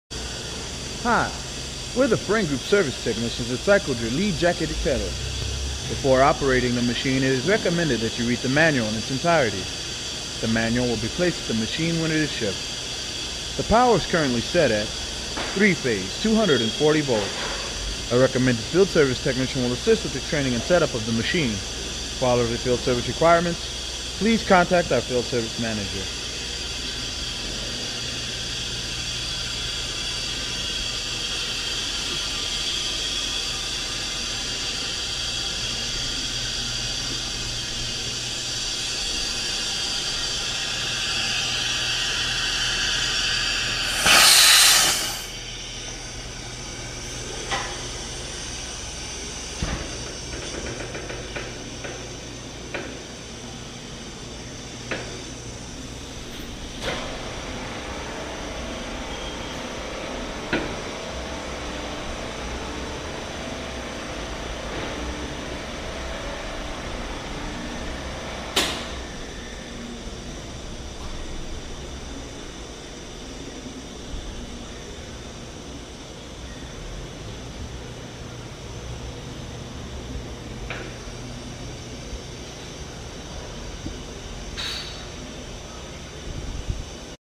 Lee 50 GAL Stainless Steel Double Motion Vacuum Kettle Demonstration
Lee, Model 50D7T, 50 gallon, stainless steel, double motion vacuum kettle. Equipped with pneumatic tilt out cover with top mounted 1 Hp anchor style scrape surface and 3 Hp / 3600 rpm Multi-Mix shear / homogenizing agitators with (2) 2" OD, (2) 2-1/2" OD sanitary top ports including sight and light ports. Full jacket rated for MAWP 90 psi at 332 Deg F. MDMT -20 Deg F at 15 psi, Internal pressure rating 15 psi at 250° F. Dish bottom with 2" OD center bottom discharge with 21" floor clearance and mounted on base frame with casters.